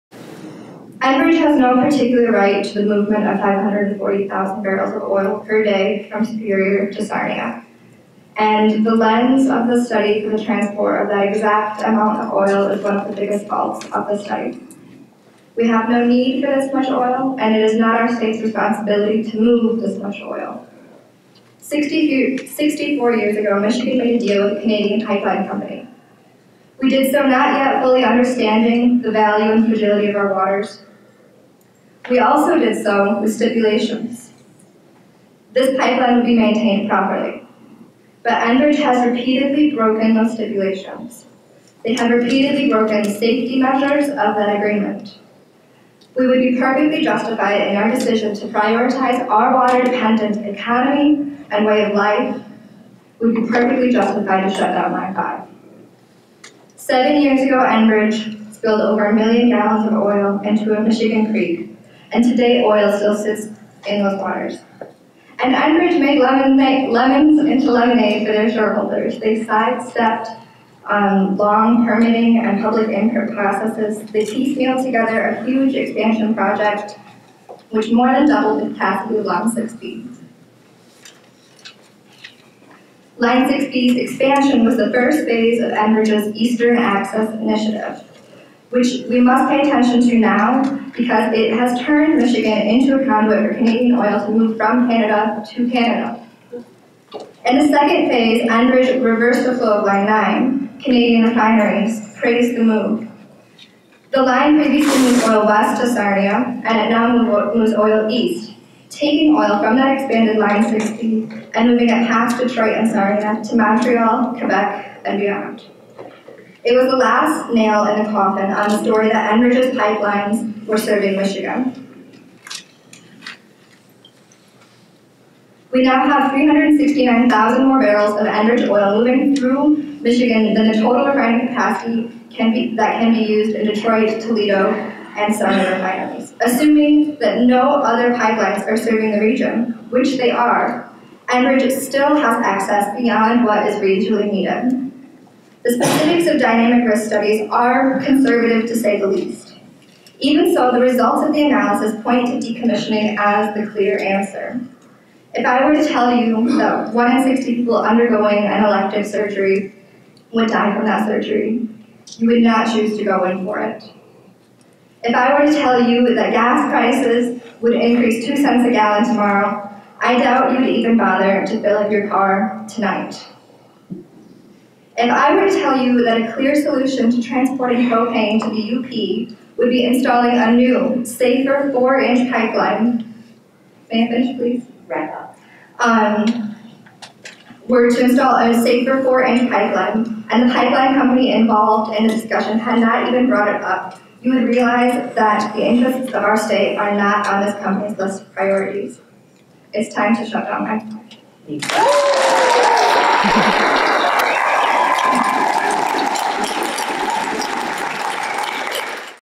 Zada Harris explains why Enbridge has no right to use the Great Lakes for oil transport
There are many reasons why we are not obligated to allow Enbridge to use the Great Lakes to transport oil, and Zada Harris hits many of them. Submit your comment to call for the decommission of Enbridge Line 5 now: